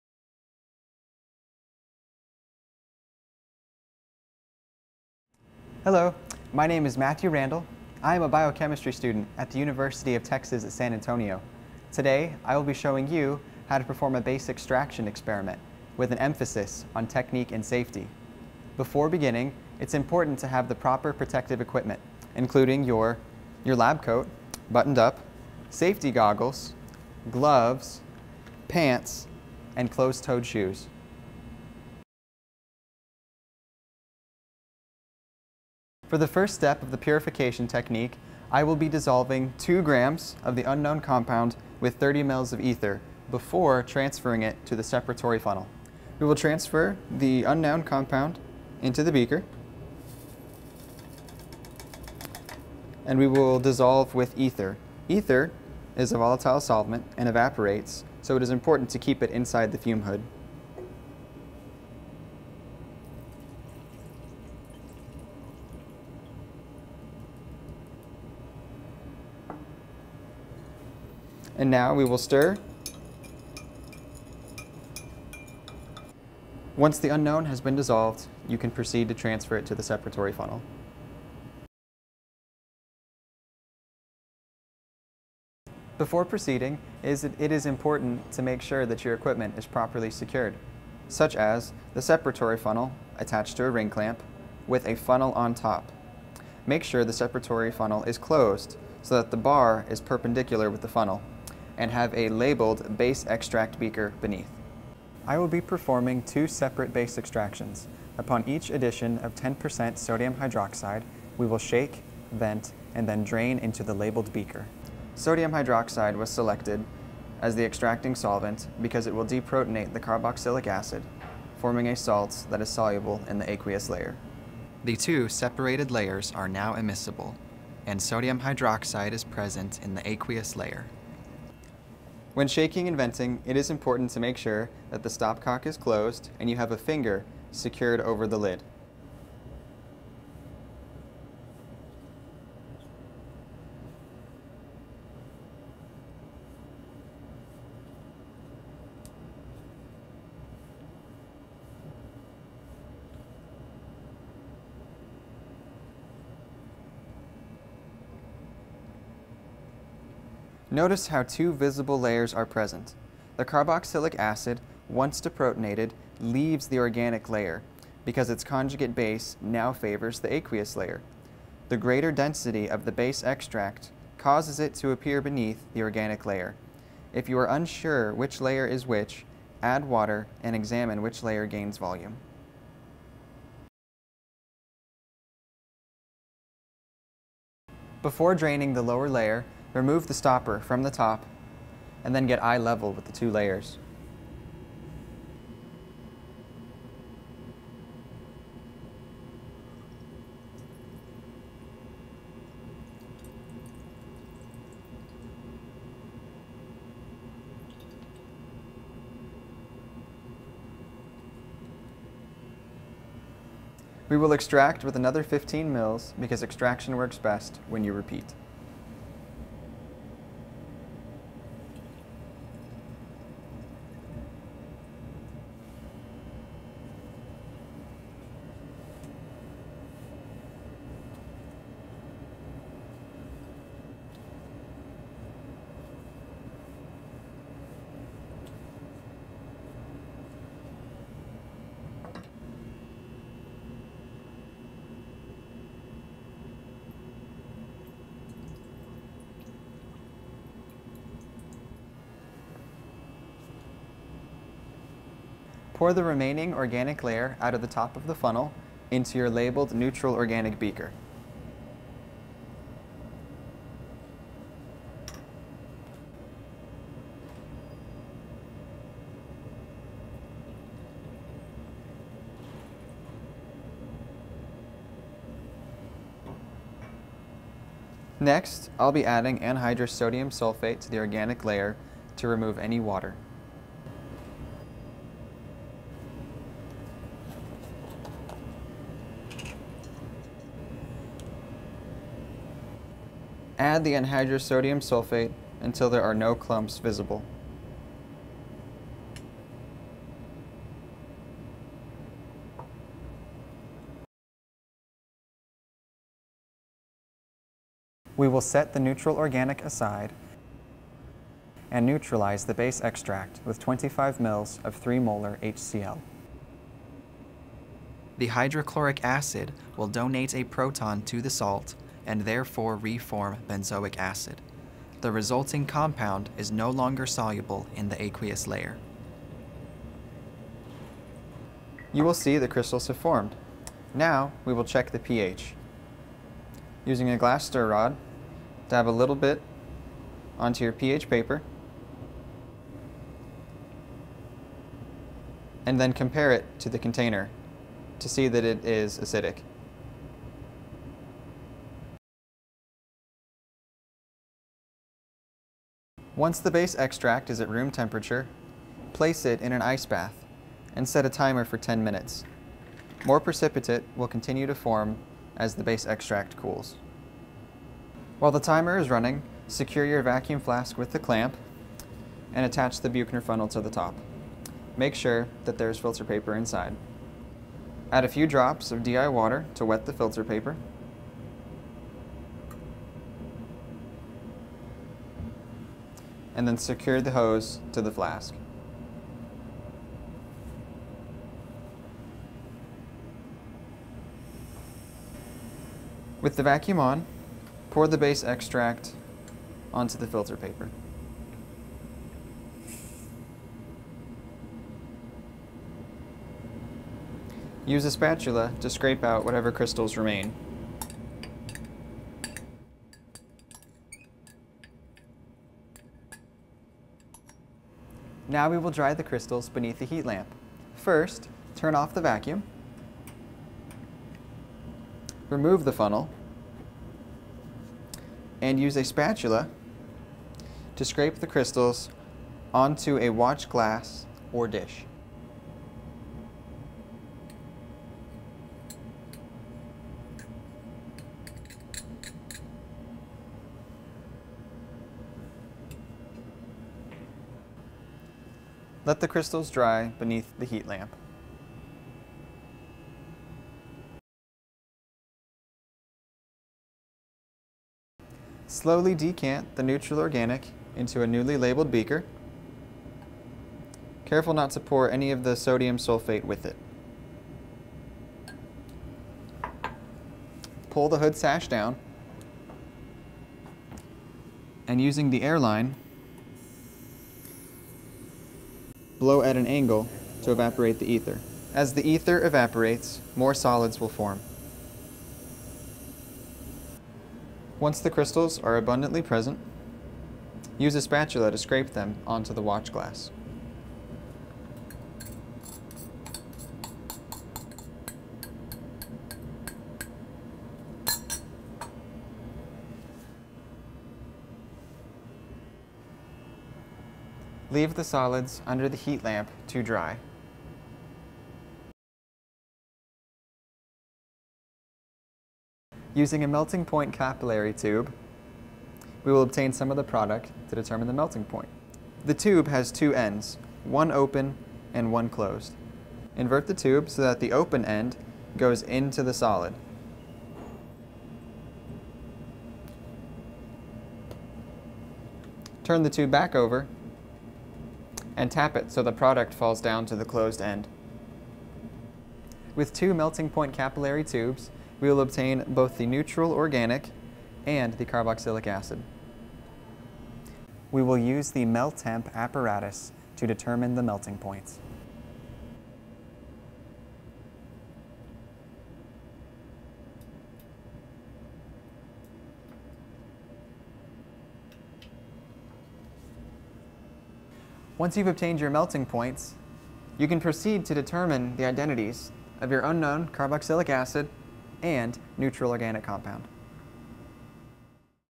UTSA Chemistry Base Extraction Demonstration
CHE 2612: Organic Chemistry I Lab - Base Extraction
Dr. Hector R. Aguilar